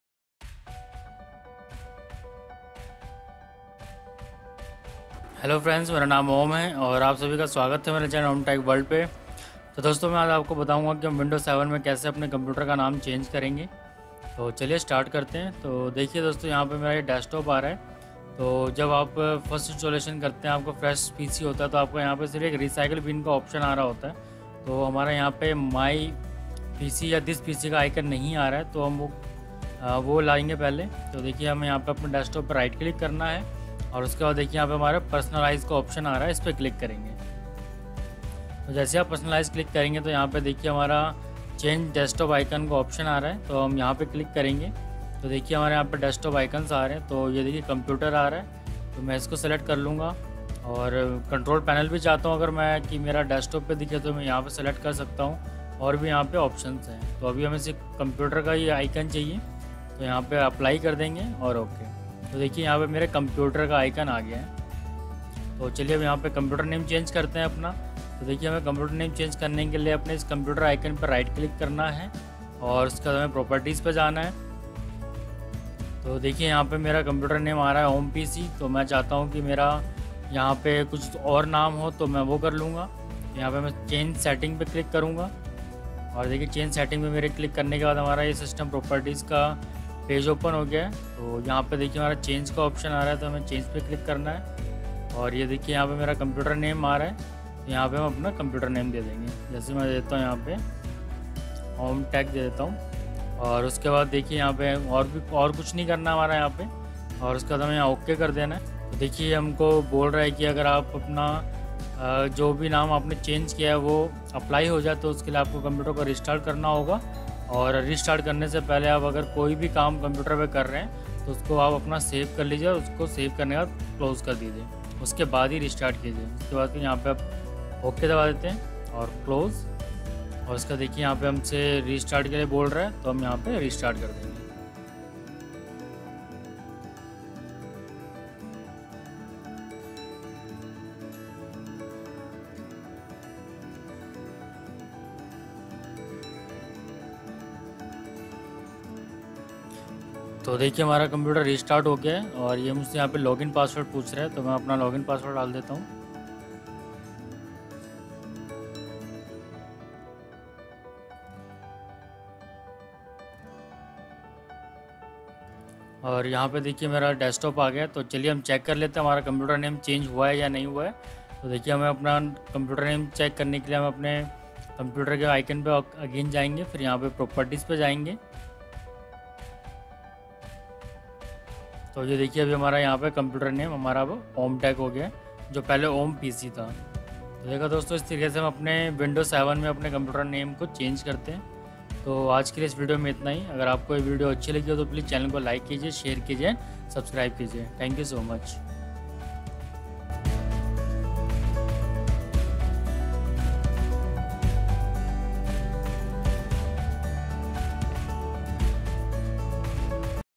How to change computer name in Window 7 Operating System
Hello Friends Maine is video mai btaya hai ki kaise hum apne computer ka name change krte hai Window 7 Operating System mai.